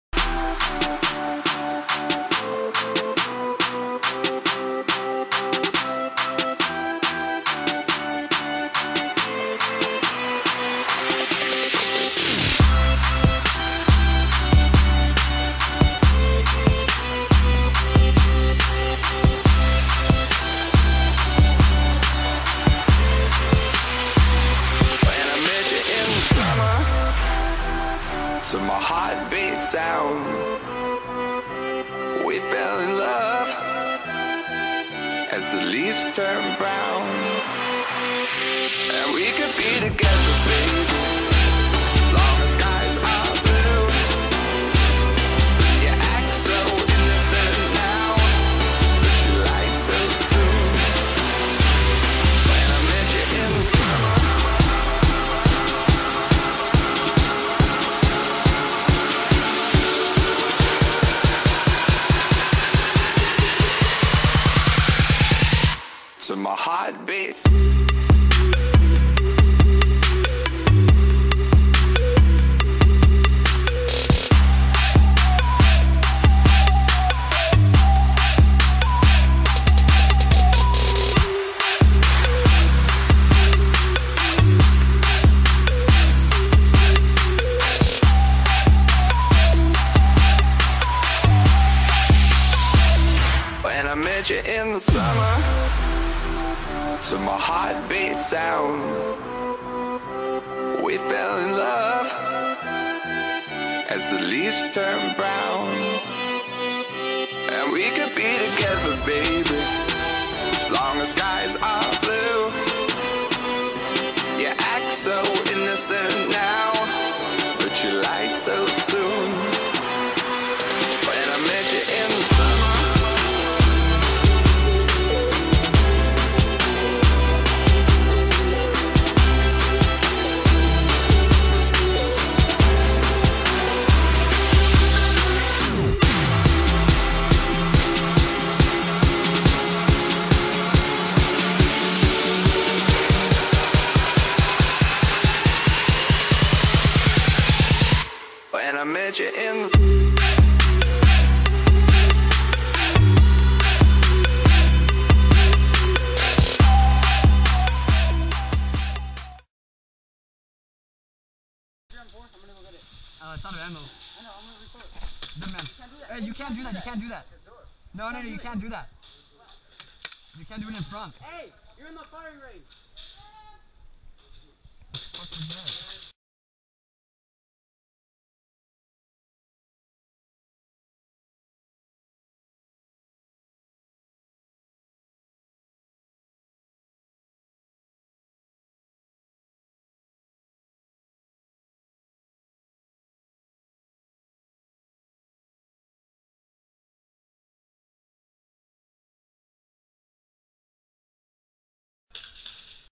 TAA Airsoft 3/5/15 Ft Cheaters and Idiots.
A bunch of stupid heads playing airsoft at a good field in MD. but the day was fun ad i wasnt about to let 3 dumbass jerks ruin my day.GG       I DONT OWN THE RIGHTS TO THE SONG